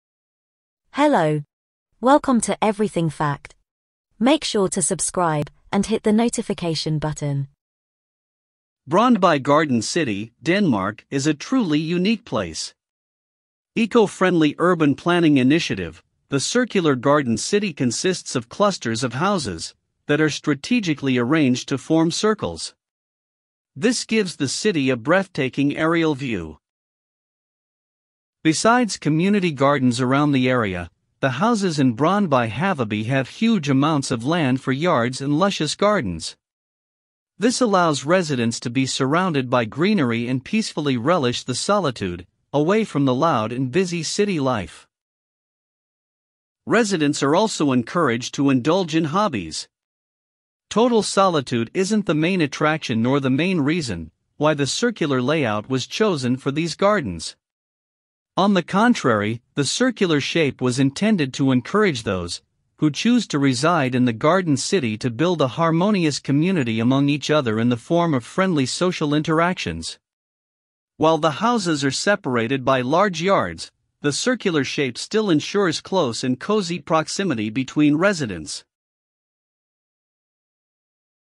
denmark garden city
On the outskirts of Copenhagen lies a curious and idiosyncratic sight; Brøndby Haveby, a collection of houses nestled inside a grid of perfectly circular greenery.